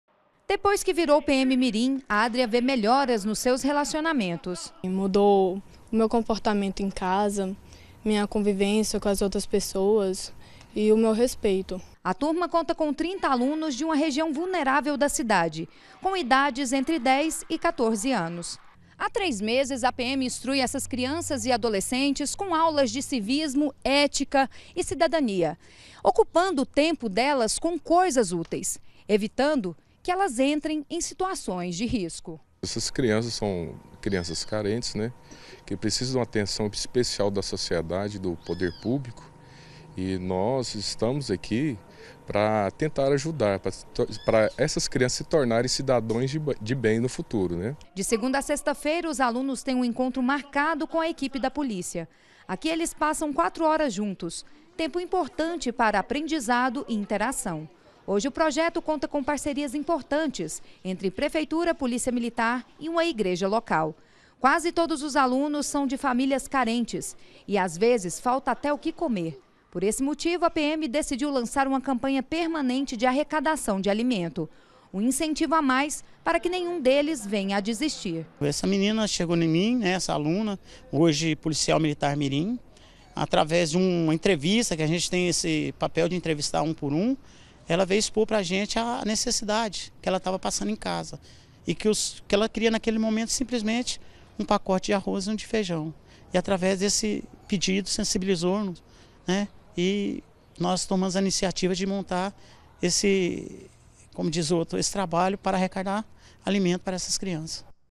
PM FAZ CAMPANHA PARA ARRECADAR ALIMENTOS E ROUPAS PARA CRIANÇAS CARENTES
Policiais militares de Anápolis começaram a fazer uma campanha para arrecadar roupas e alimentos para crianças carentes que fazem parte do projeto PM mirim. Os militares se solidarizaram depois que uma aluna relatou para um professor que estava passando fome em casa.